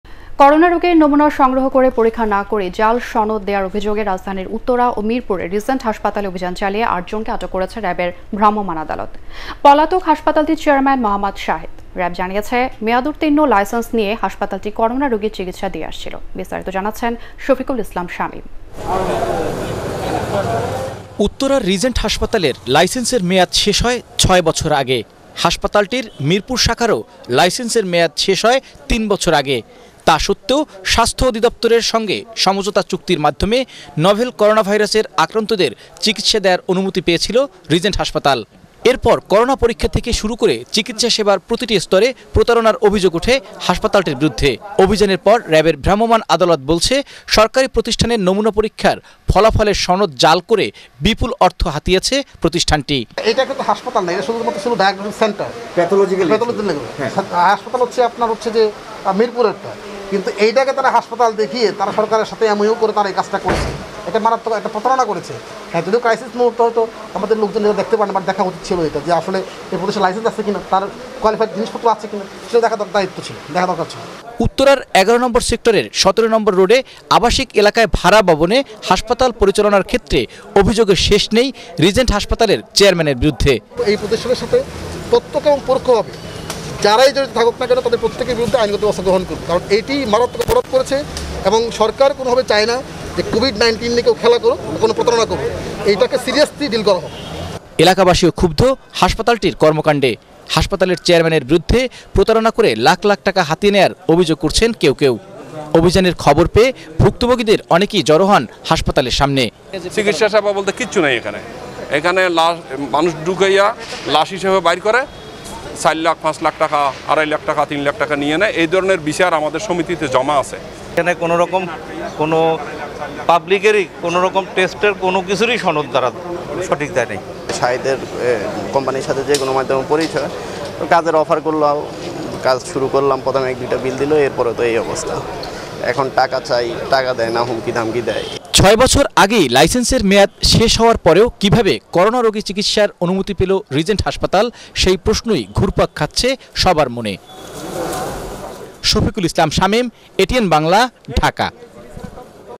করোনার ভুয়া রিপোর্ট দেয়ার অভিযোগে রাজধানীর রিজেন্ট হাসপাতালের দুই শাখায় র‌্যাবের অভিযান।
নমুনা পরীক্ষা না করে করোনার ভুয়া রিপোর্ট দেয়ার অভিযোগে রাজধানীর রিজেন্ট হাসপাতালের দুই শাখায় র‌্যাবের অভিযান। আটক ৮।